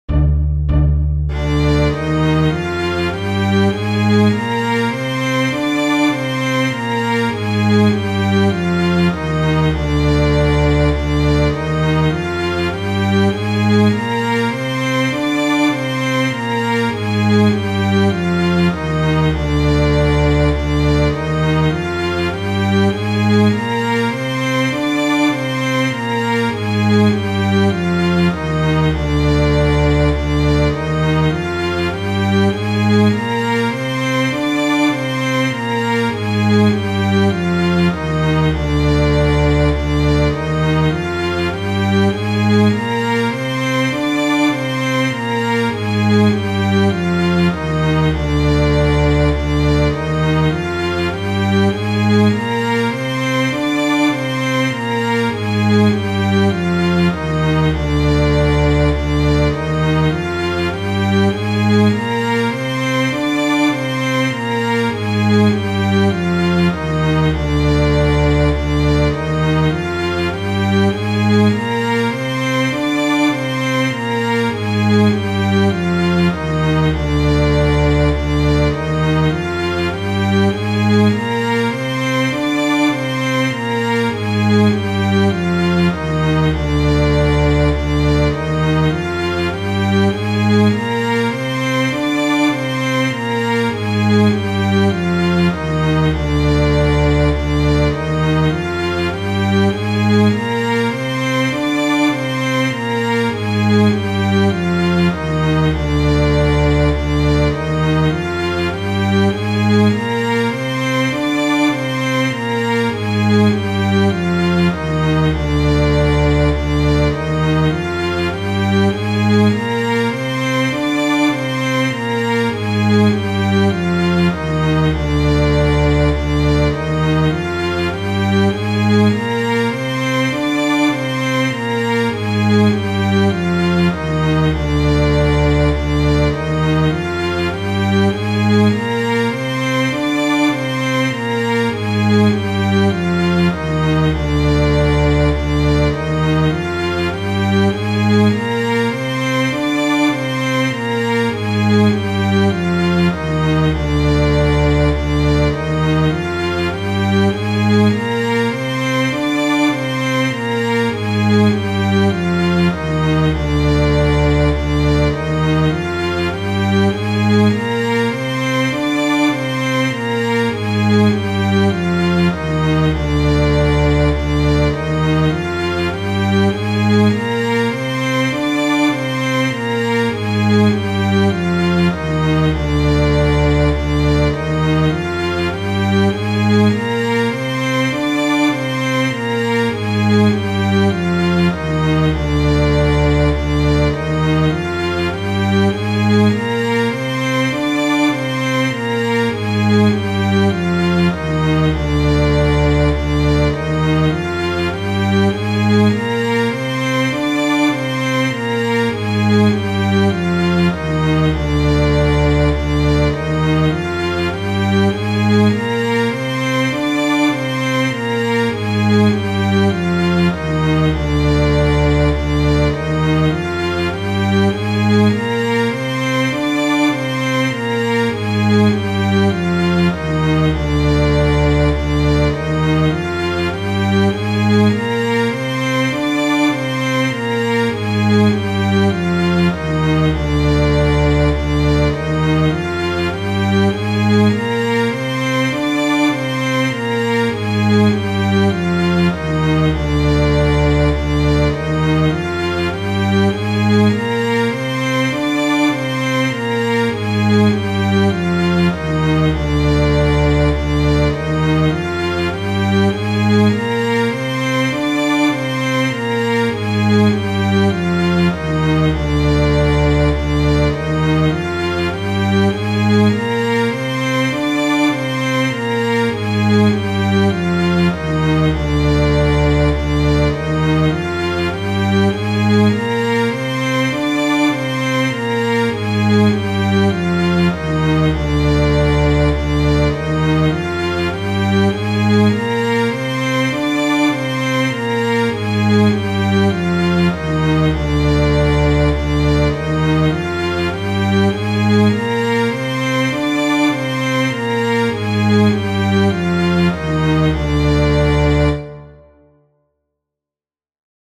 D Locrian Scale One Octave at 100bpm Backing Track Color
Play or sing along. This track features double bass, cello, viola and violin playing D Locrian Scale One Octave at 100bpm. Sheet music is provided on G treble clef, C alto clef and F bass clef.